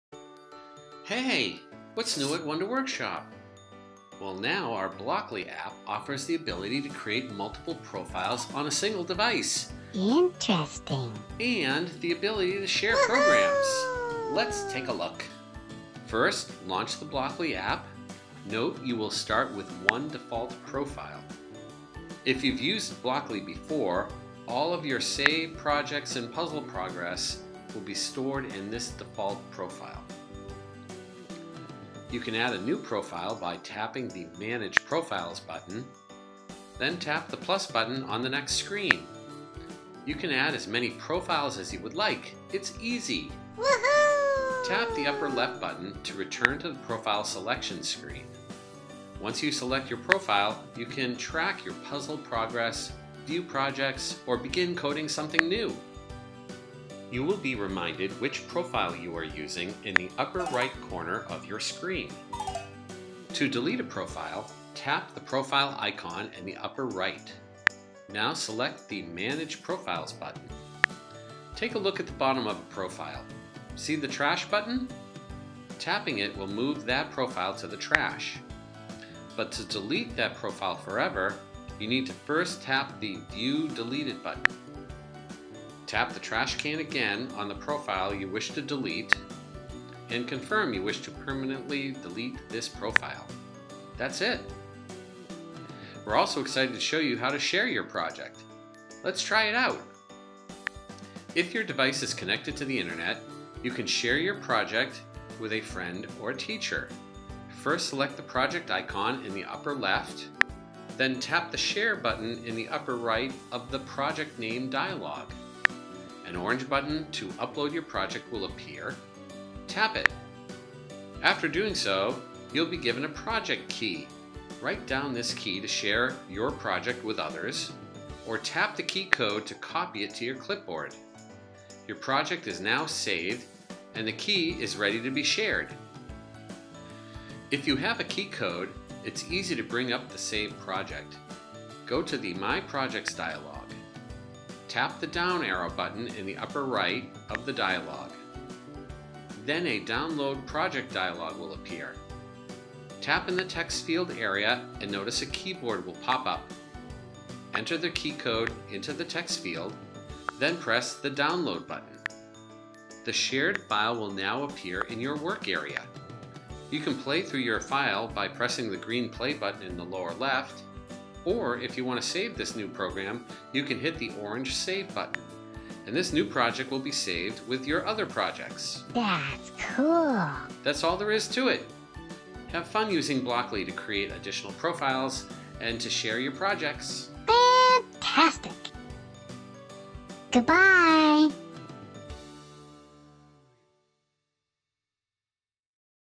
Blockly App - Profiles & Sharing | Wonder Workshop
Find out how to create multiple profiles on a single device, and the ability to share programs. Personalize an individual profile from one of 20 different avatars and seven background colors. Find out how to share and download Blockly projects using Project Keys. Have fun using Blockly to create additional profiles and to share your projects!